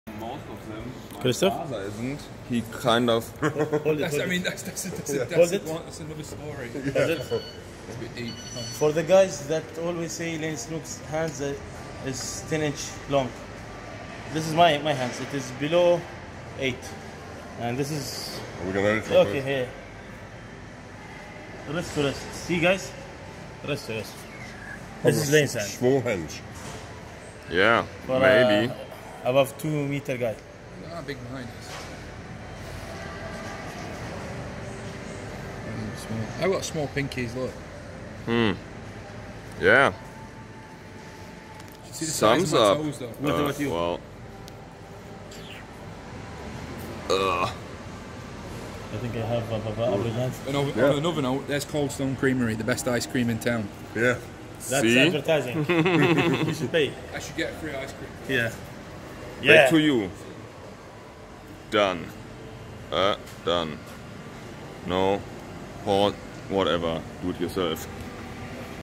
Hand comparison me and Laine Snook
This is for the guys who says Laine Snook's hand is more than 10 inches hand. Heck one guys posted on the gripboard that Mark Felix has 15 inch hands and Laine Snook has 11.5 Inch hands. My hands aren't large at all. I have decent grip. Here is my hand size comparing to Laine, then James Ashley Garrad, and finally Christoph Koch.